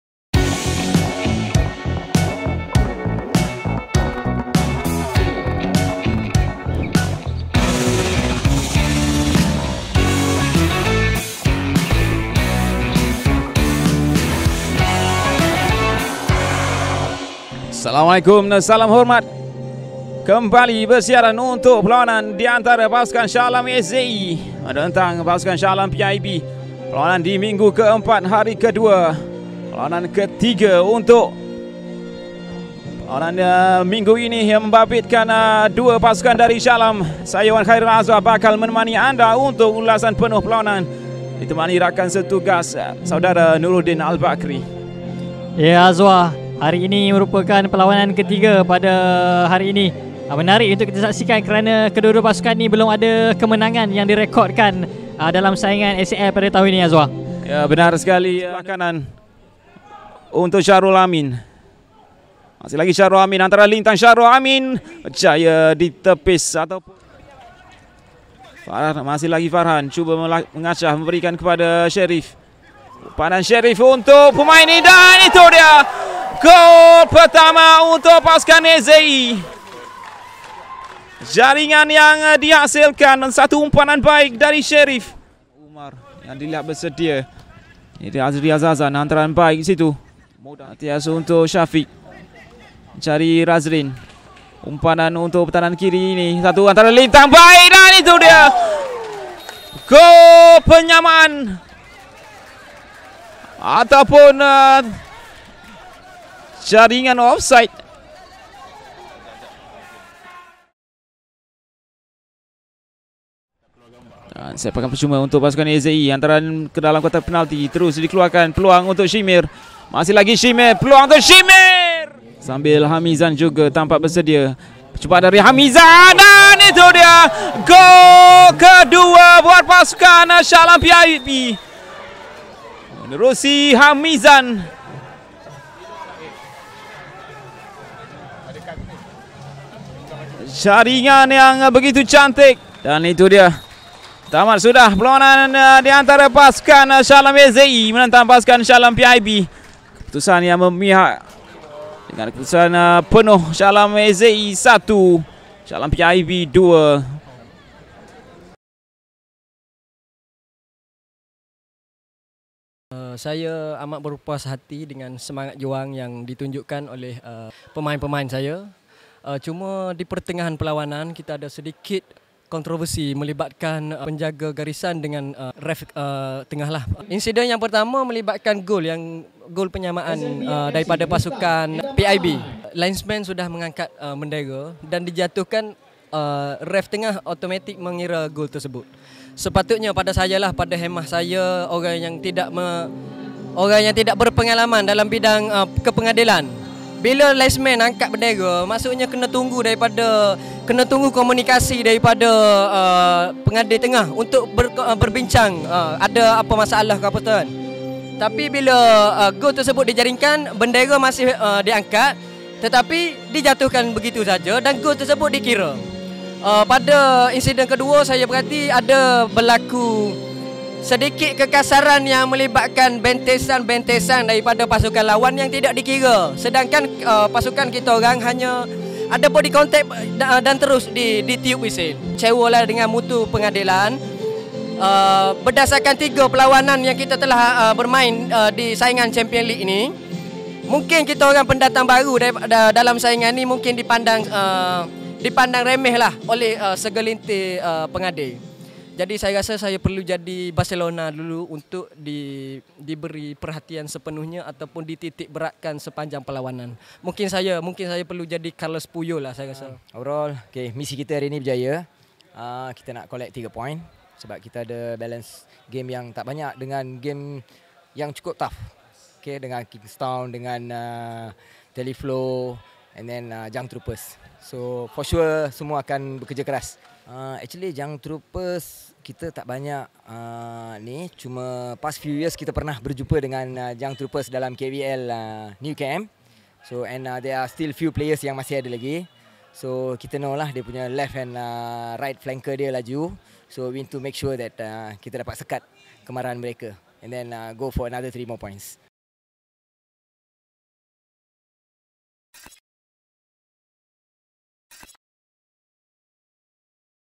SCL2019 | MW4 | Group A | SA AZE FC 1 - 2 SA PIB FC
Selangor Champions League 2019 Matchweek 4 | Group A

SA PIB FC berjaya mengumpul 3 mata yang berharga dalam kumpulan A selepas menewaskan SA AZE FC 2-1 semalam di Stadium Arena MPSJ, Subang Jaya.